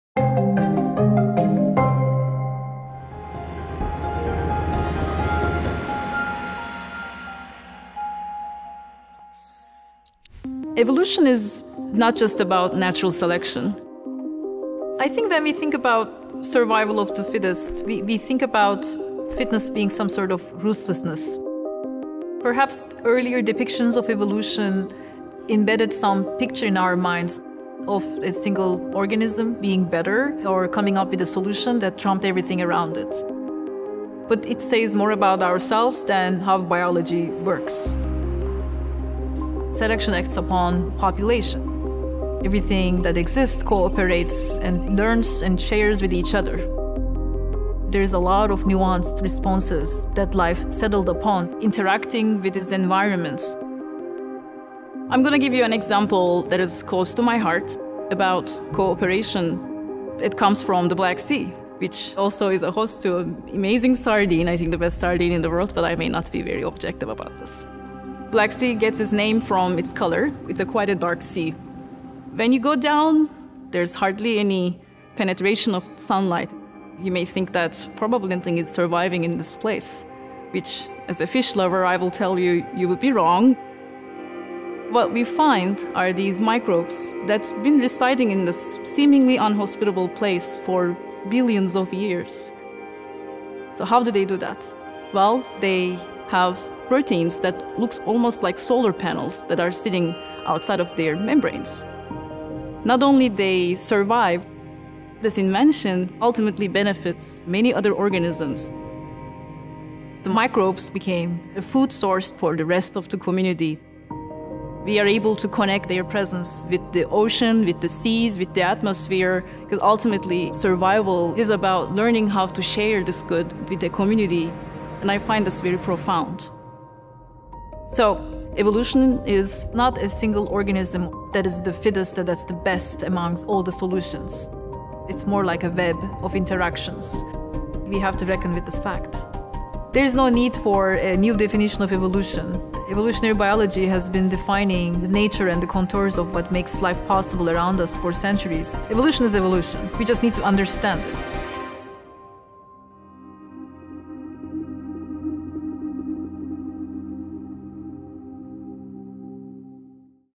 The ocean is evolving, and it’s not based on the ‘survival of the fittest’ | Betül Kaçar
“Ultimately, survival is about learning how to share with the community.” Astrobiologist Betül Kaçar on how true evolution relies on collaboration, not competition.

What if evolution isn’t just about the strongest winning?

Astrobiologist Betül Kaçar explains that life survives by working together, not just by competing. To elaborate, she uses an example about microbes in the Black Sea that help other organisms by sharing energy. True evolution, she says, is not about being the best alone, it’s about connection, cooperation, and learning to live together.